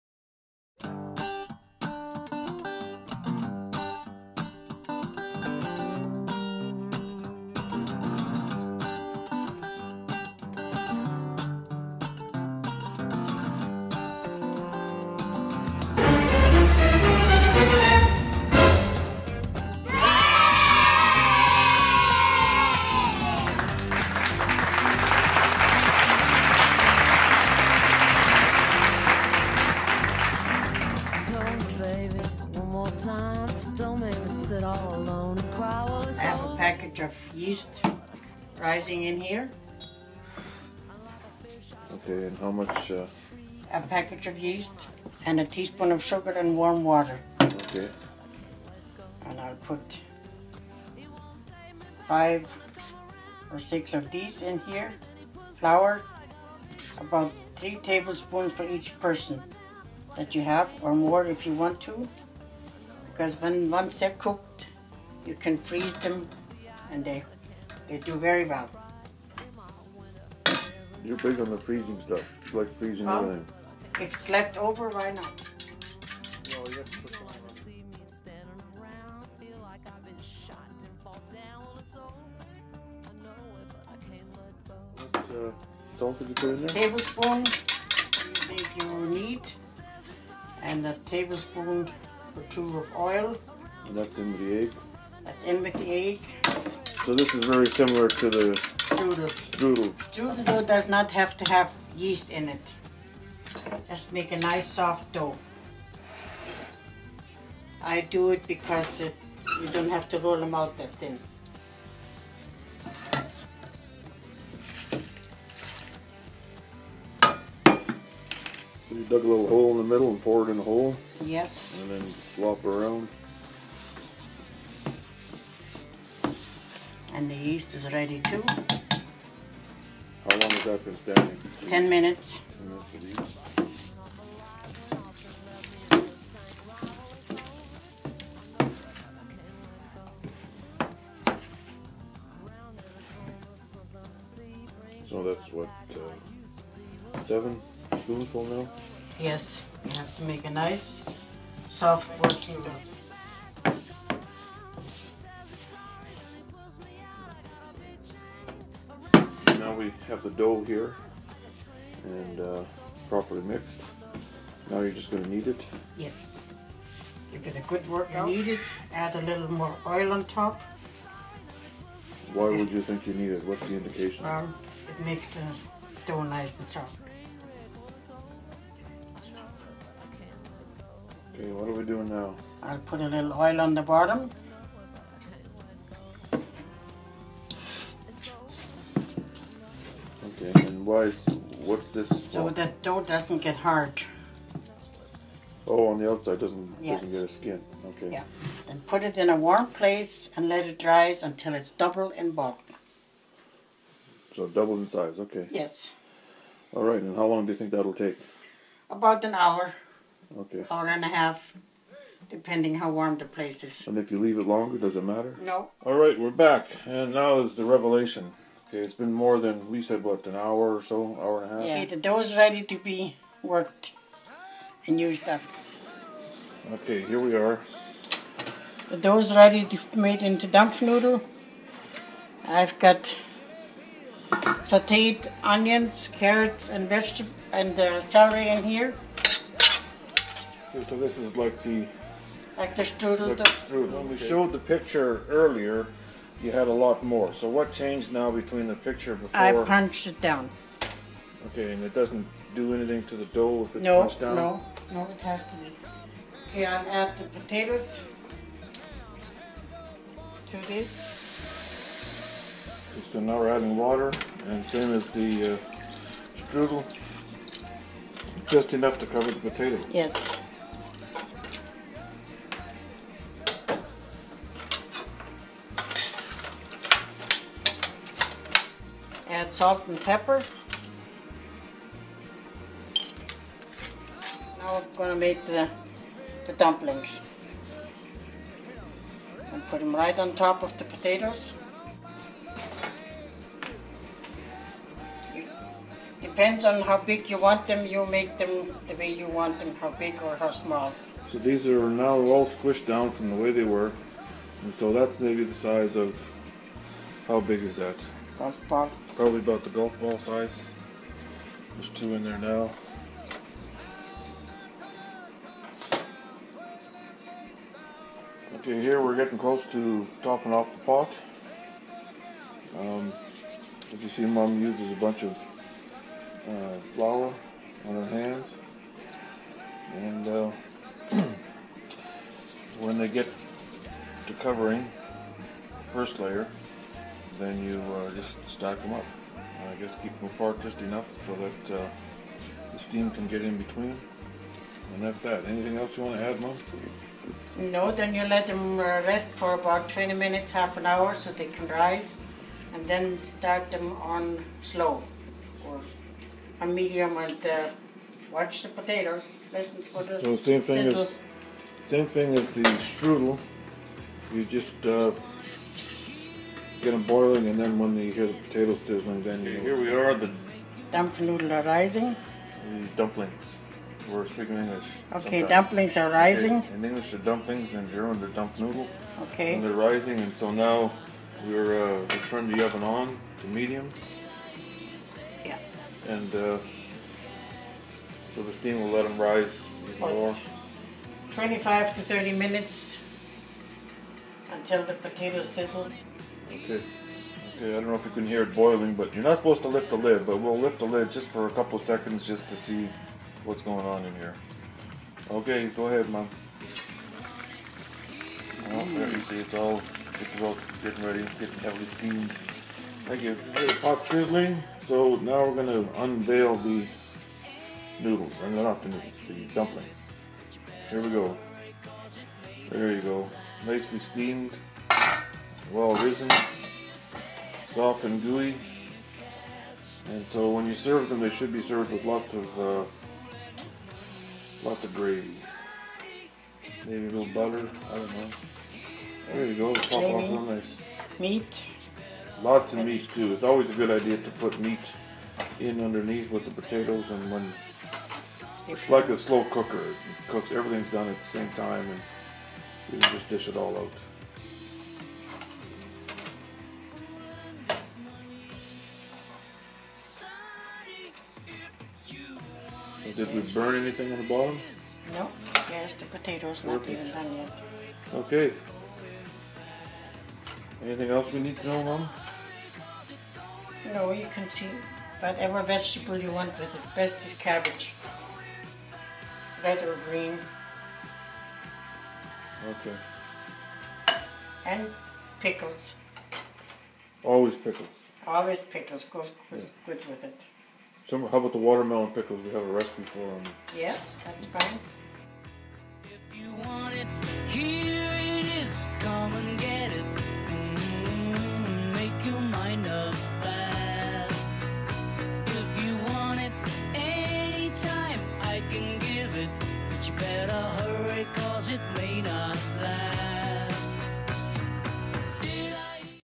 The Dumpling Manifesto
This is a dampfnudel (steamed nodel), or dumpling recipe made by my mom and grandma.  Wanted to capture some heritage before it disappeared. Did it all without a government grant too!!